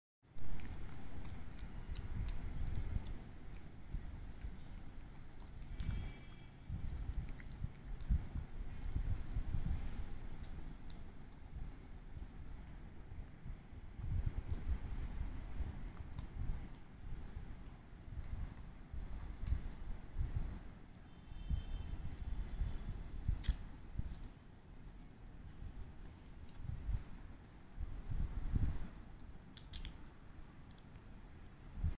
Cocker Spaniel Pup chewing her favorite bone! (ASMR 😁)
Rumi enjoying her favorite bone and showing off her dangling beautiful ears!! Such a vain puppy I tell you! 😁😁